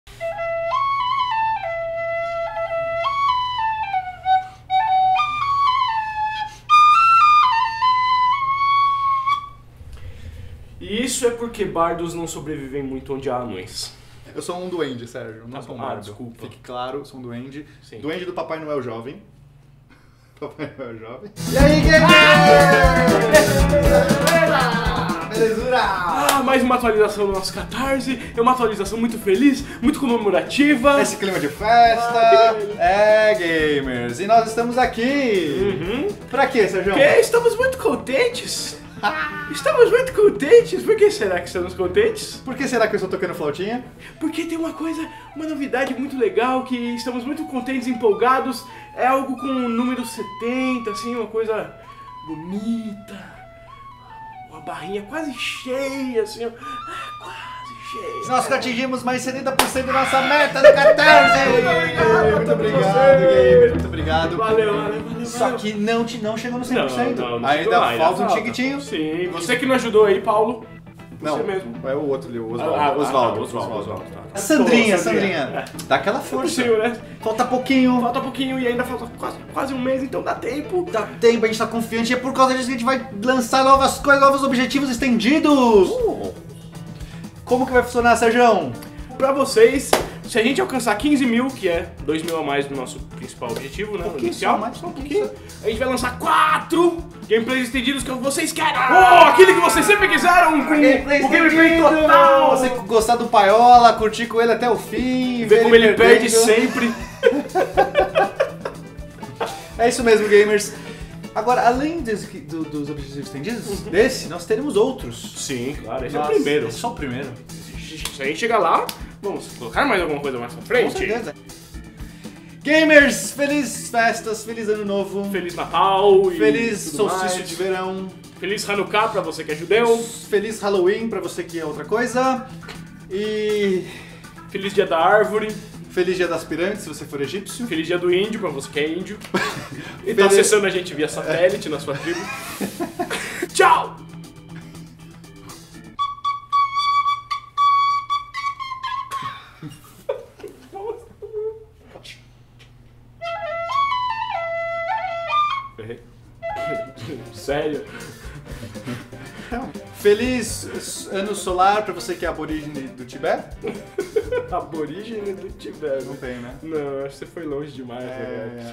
Update Catarse - Ho Ho Ho - Novidades
Estamos chegando lá gamers. Para comemorar, anunciamos nosso primeiro objetivo estendido.
Compartilhe, colabore!

Muito obrigado a vocês.